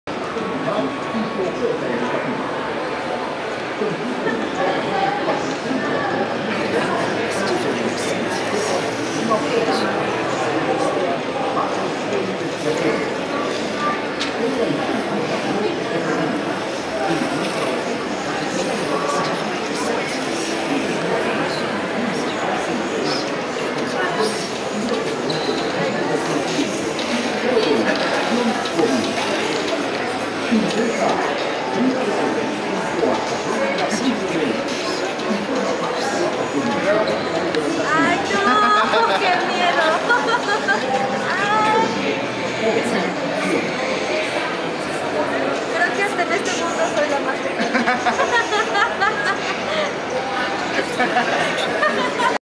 video recorrido virtual Academica FIL 1
Dos visitantes de la FIL experimentan un recorrido virtual por el Museos Soumaya y por paisajes del mundo en el Stand de Académica.